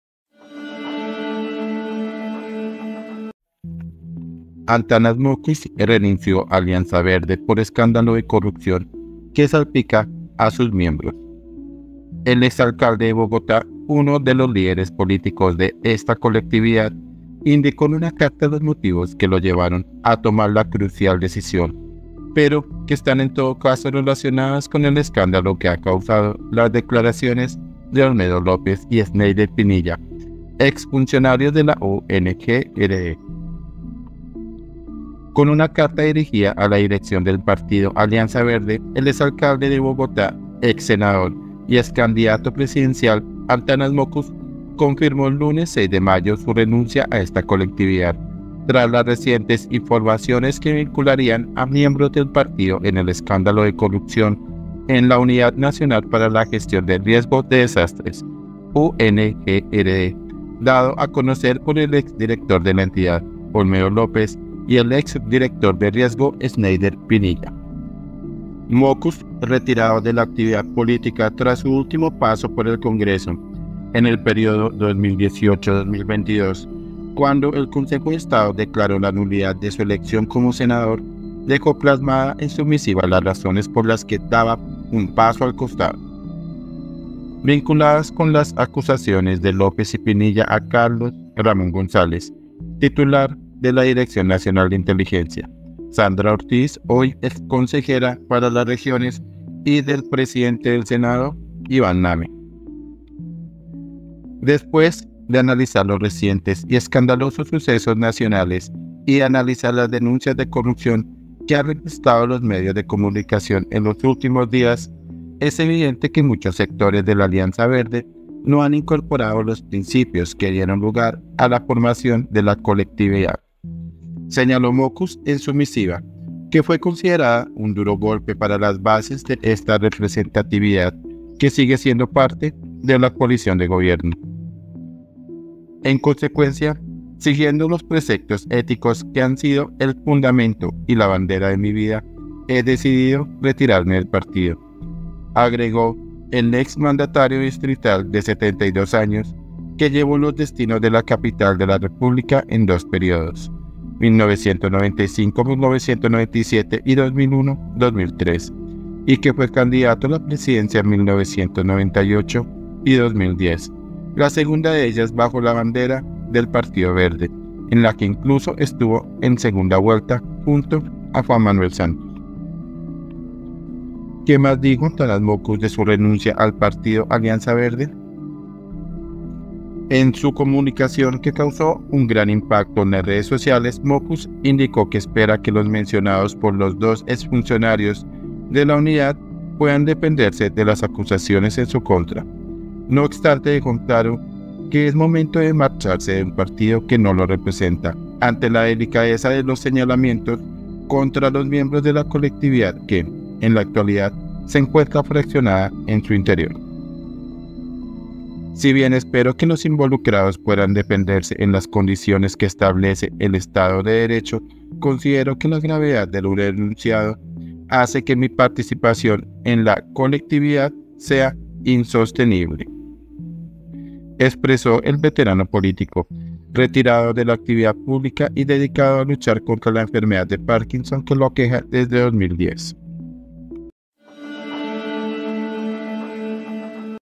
🟢 ANTANAS MOCKUS RENUNCIÓ A ALIANZA VERDE POR ESCÁNDALO DE CORRUPCIÓN QUE SALPICA A SUS MIEMBROS
El exalcalde de Bogotá, uno de los líderes políticos de esta colectividad, indicó en una carta los motivos que lo llevaron a tomar la crucial decisión, pero que están en todo caso relacionados con el escándalo que han causado las declaraciones de Olmedo López y Sneyder Pinilla, exfuncionarios de la Ungrd
INFOBAE